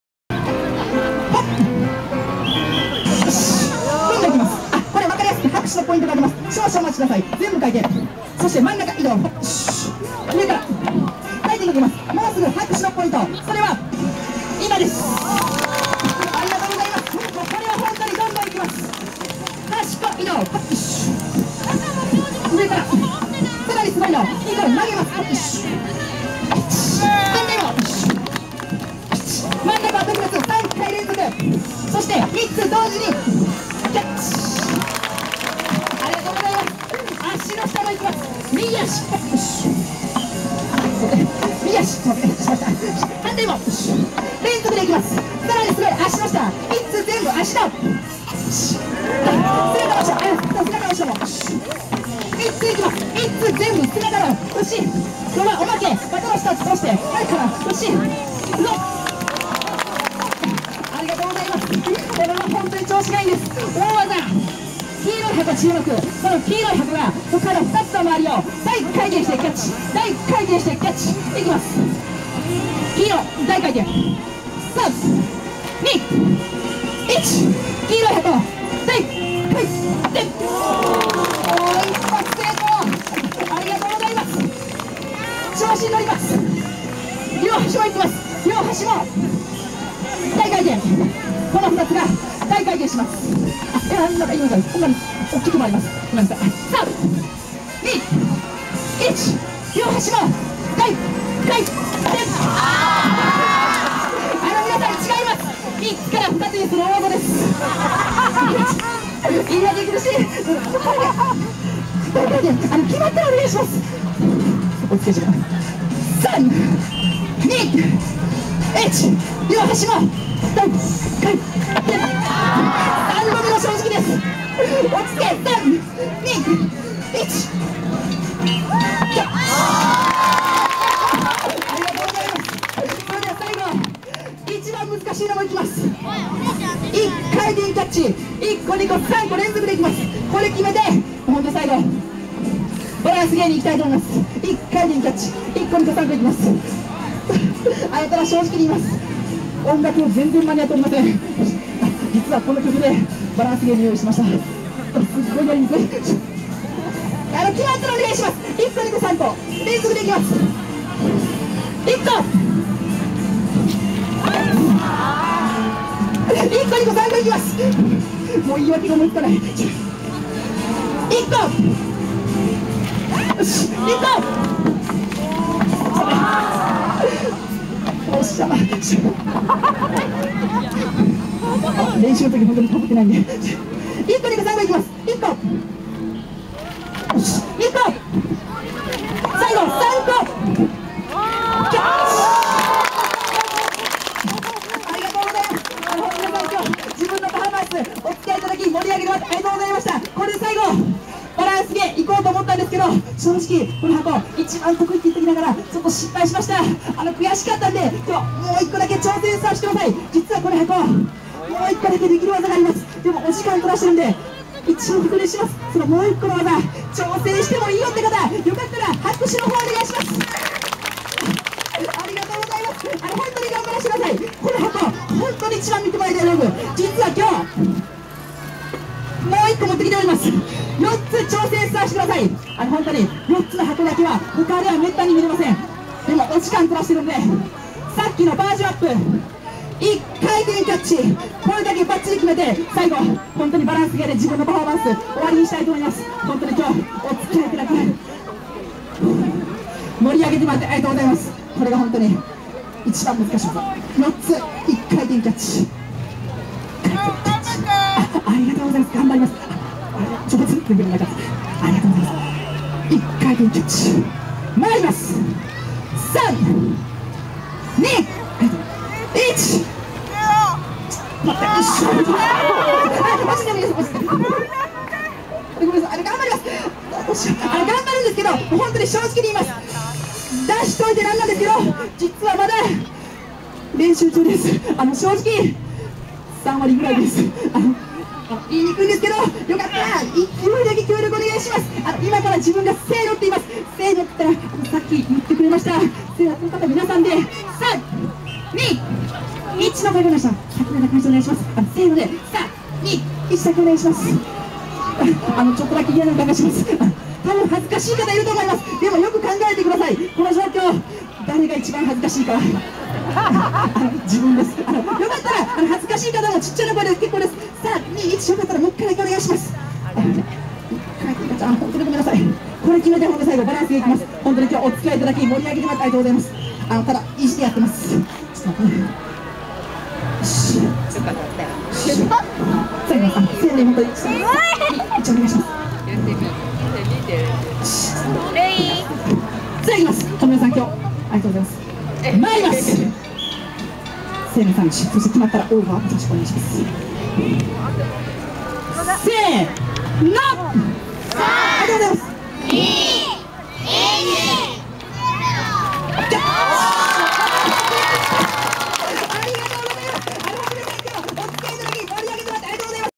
大道芸人・2011生野まつり・・Ⅲ
２０１１年大阪の生野区の生野まつりで、
大道芸人を見ました。